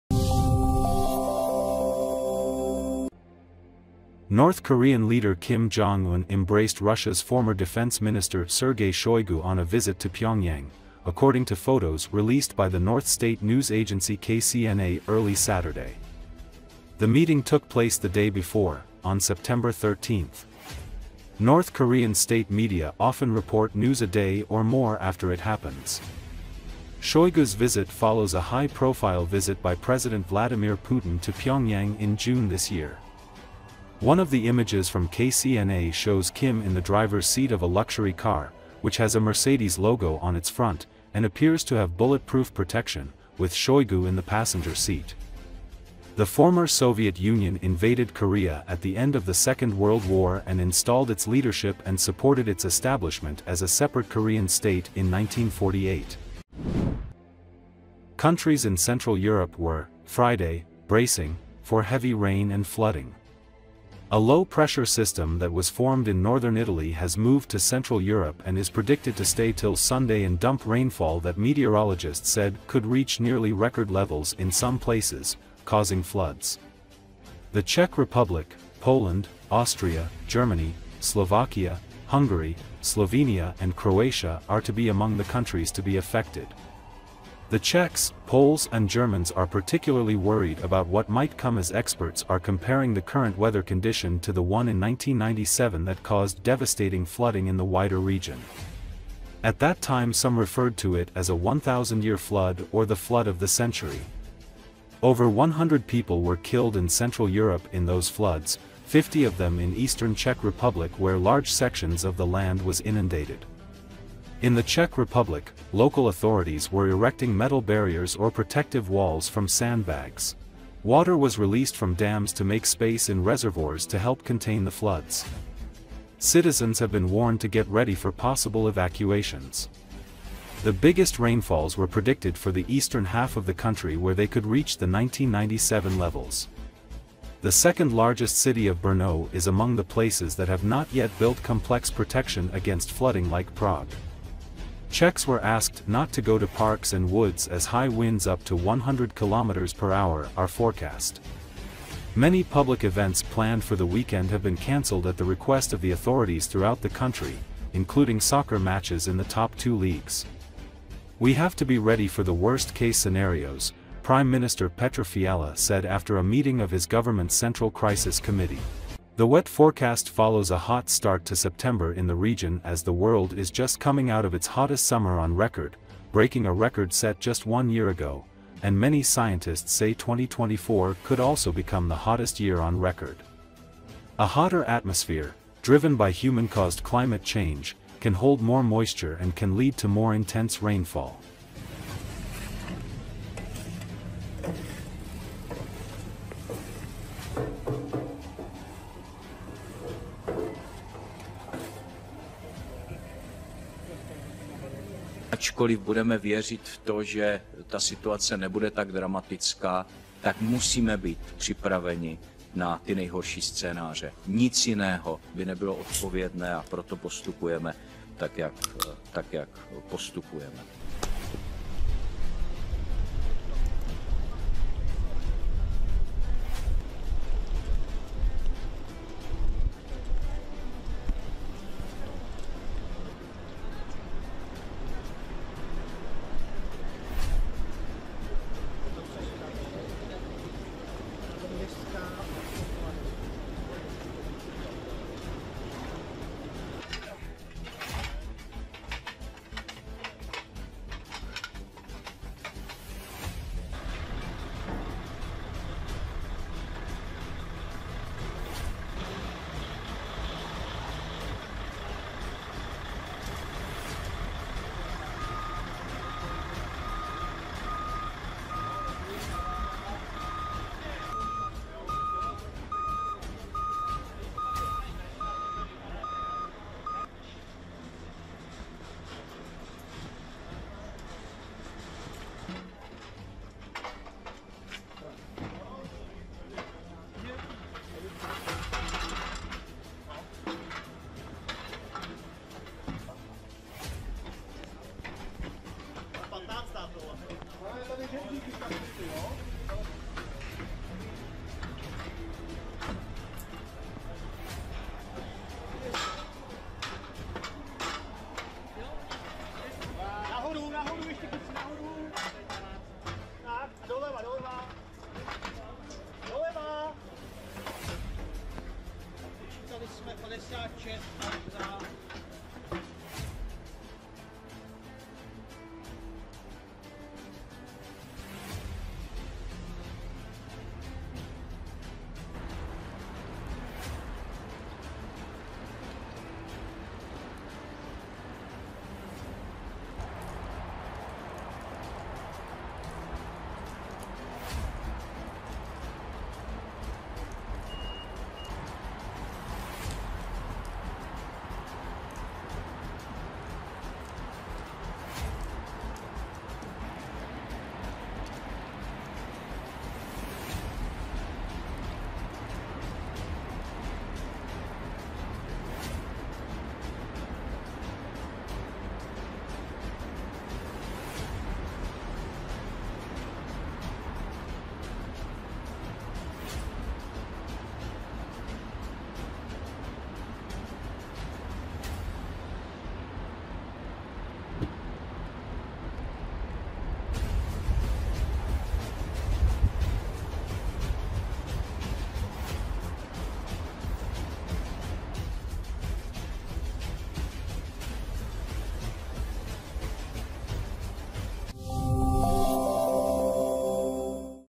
North Korea's leader Kim Jong Un meets Russia’s security council secretary in Pyongyang
North Korean leader Kim Jong Un embraced Russia's former Defence Minister Sergei Shoigu on a visit to Pyongyang, according to photos released by the North's state news agency KCNA early Saturday.

The meeting took place the day before, on 13 September. North Korean state media often report news a day or more after it happens.

Shoigu's visit follows a high profile visit by President Vladimir Putin to Pyongyang in June this year.

One of the images from KCNA shows Kim in the driver's seat of a luxury car, which has a Mercedes logo on its front, and appears to have bulletproof protection, with Shoigu in the passenger seat.

The former Soviet Union invaded Korea at the end of the Second World War and installed its leadership and supported its establishment as a separate Korean state in 1948.